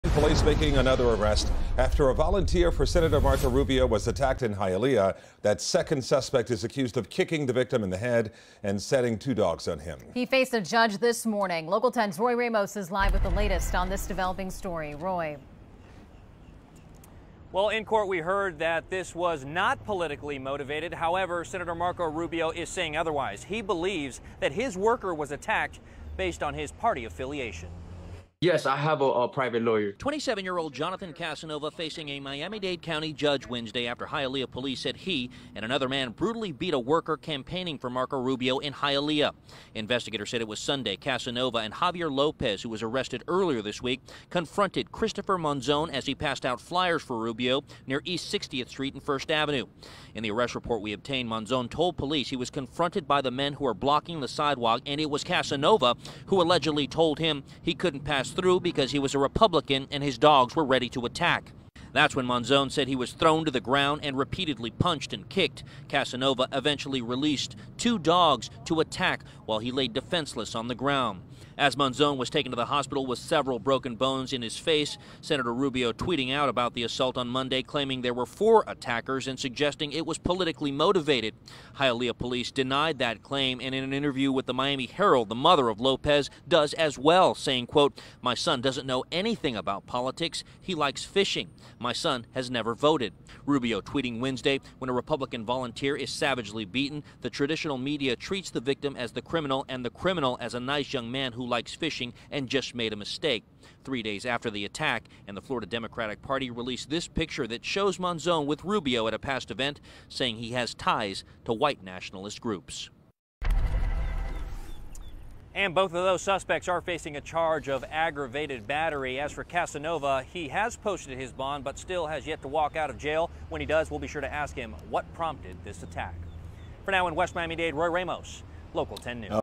Second man arrested in attack on Marco Rubio canvasser faces judge
Police announced the arrest of a man they say participated in the attack of a volunteer for Republican Sen. Marco Rubio’s campaign in Miami-Dade County.